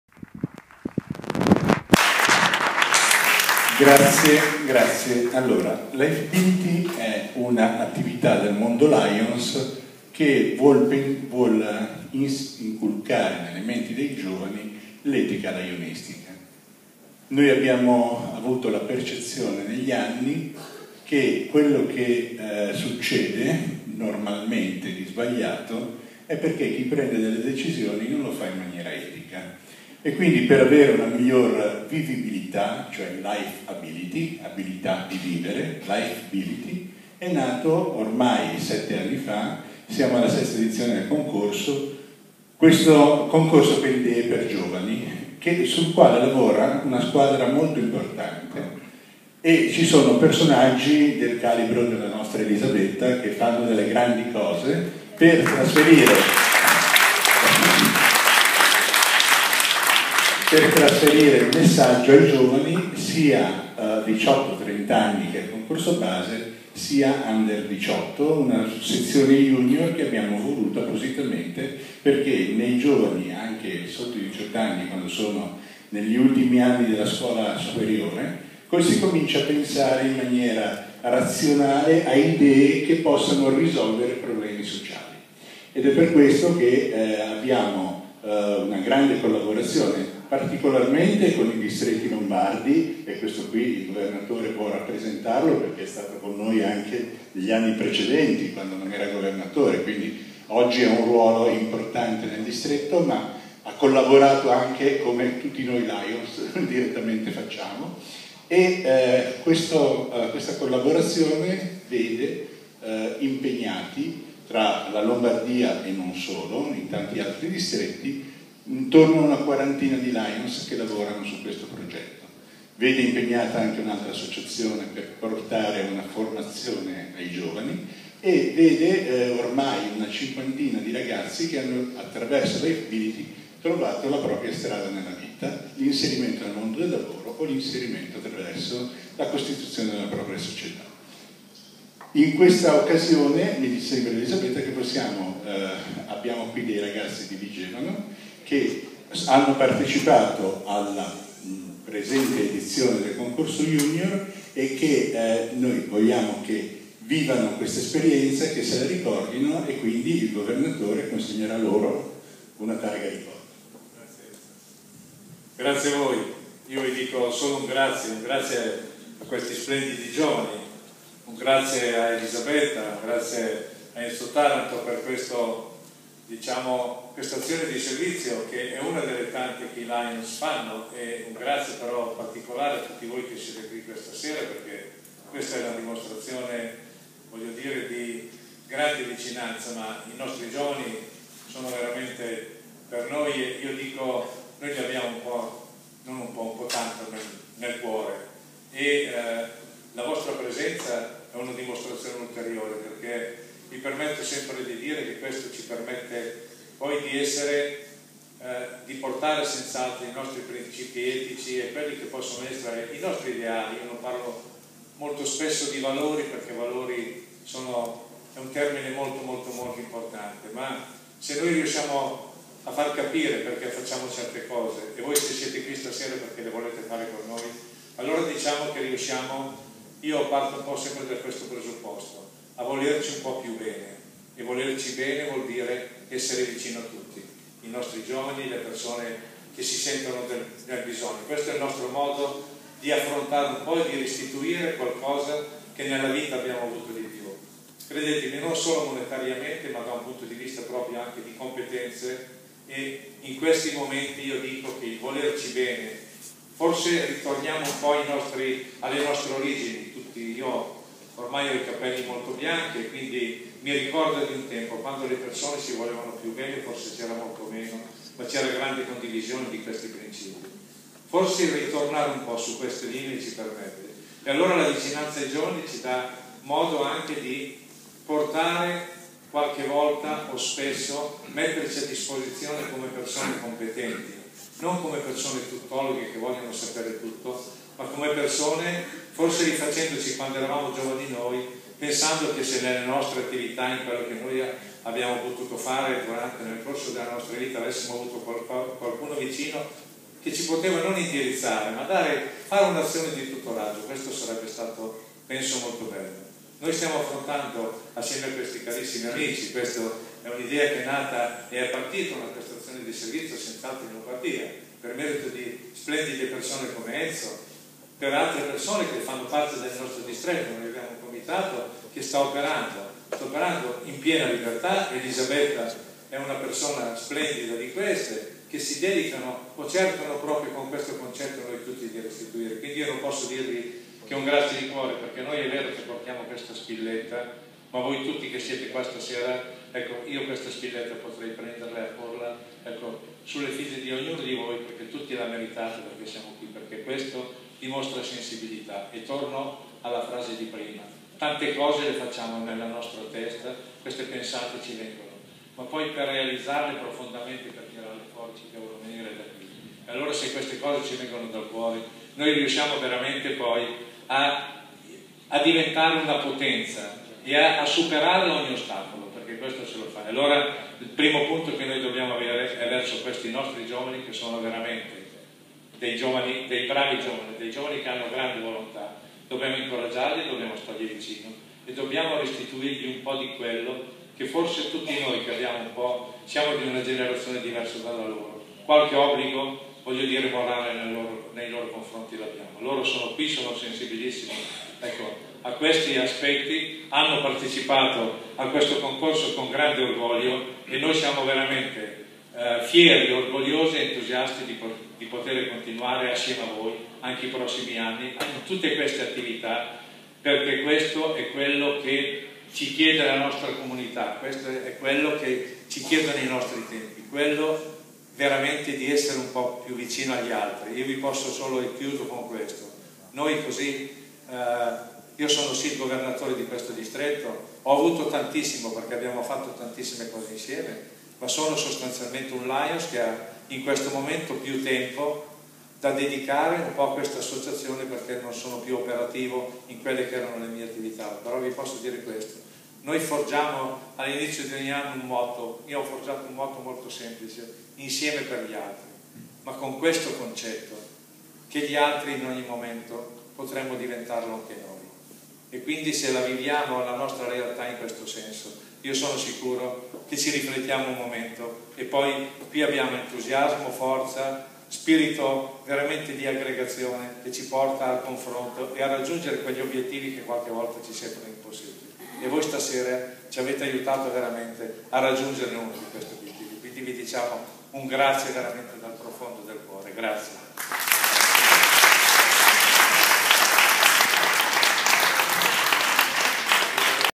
Mortara 14/05/2016 Discorso di Enzo Taranto e Massimo Alberti sul concorso Lifebility Award
Durante la serata musicale svoltasi a Mortara il 14/05/2016 per il concorso Lifebility Award, il presidente dell'associazione, Enzo Taranto presenta il concorso e il Governatore dei Distretti Lombardi, Massimo Alberti, fa un discorso di ringraziamento al comitato Lifebility per l'impegno profuso della diffusione di idee sociali e eticamente corrette.